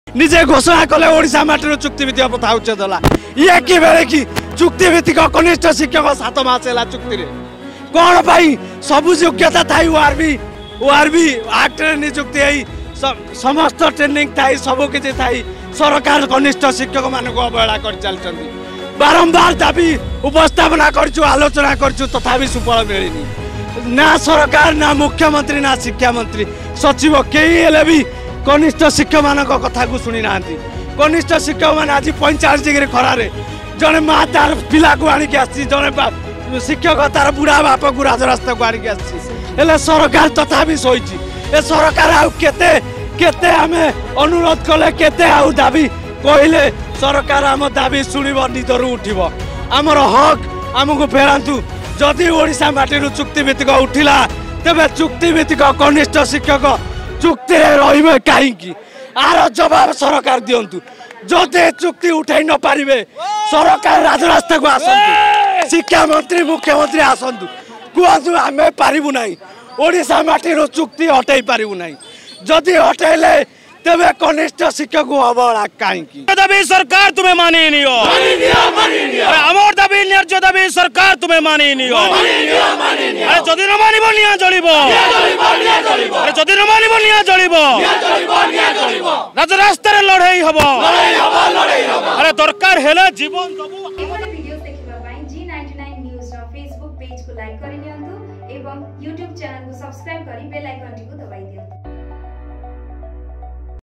High School CBT Teachers Association Protest at Lower PMG in Bhubaneswar || G99News
Connect With G99News Live Other Digital  Platform